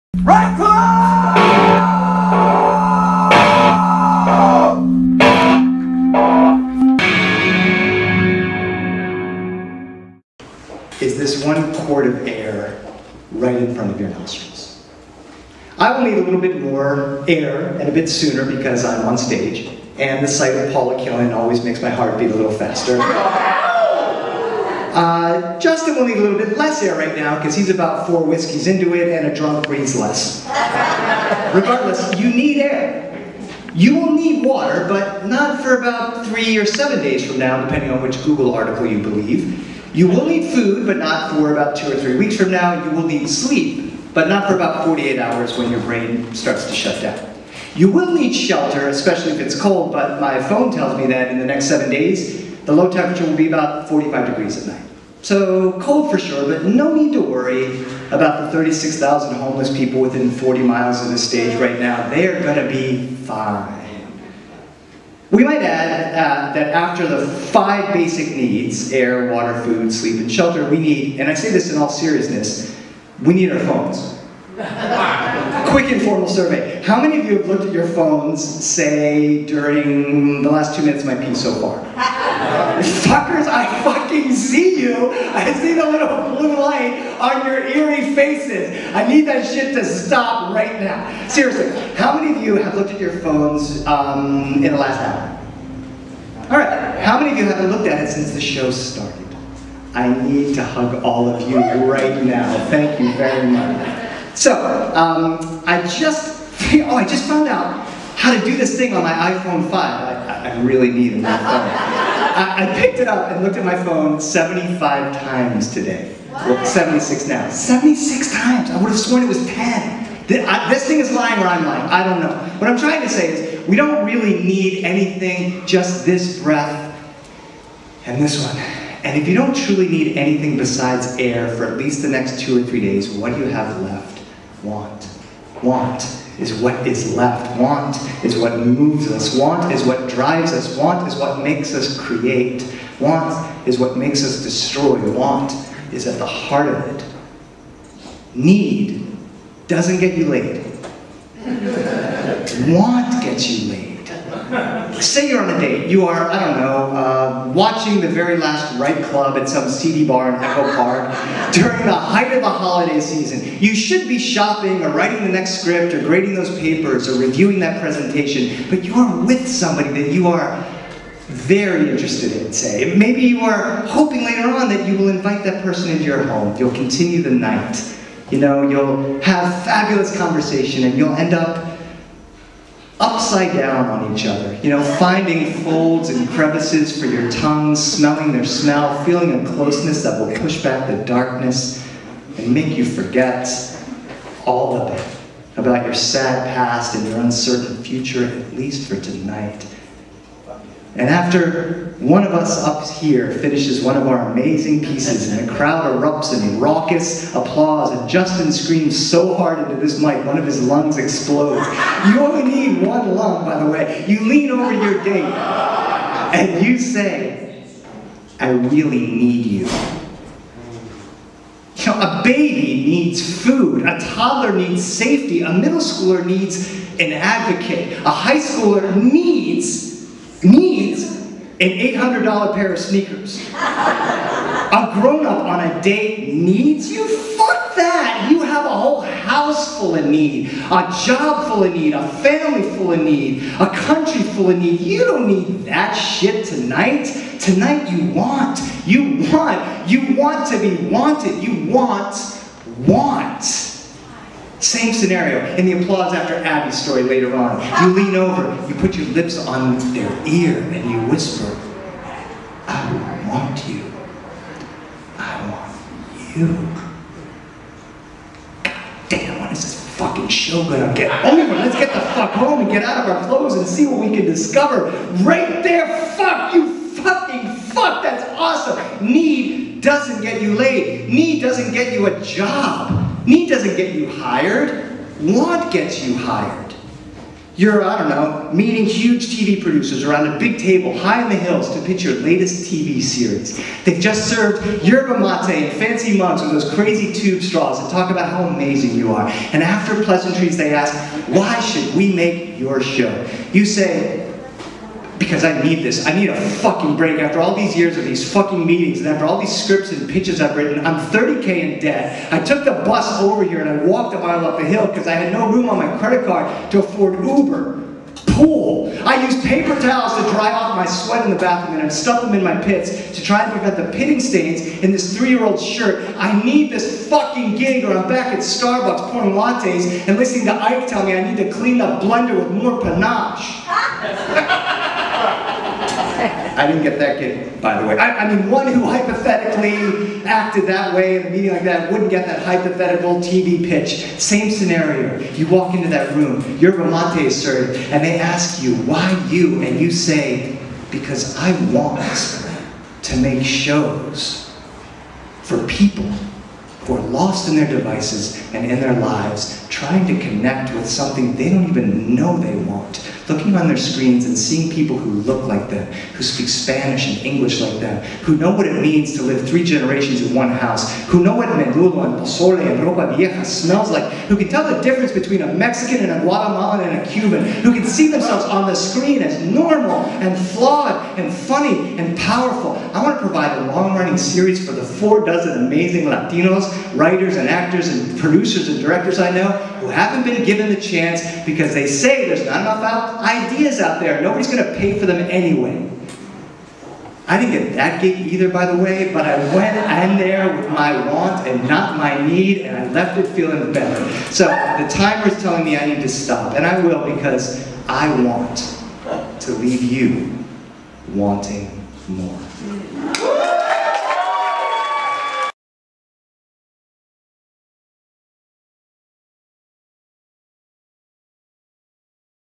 WRITE CLUB LOS ANGELES -ANTONIO SACRE
Antonio Sacre fights for 'Want" over Need.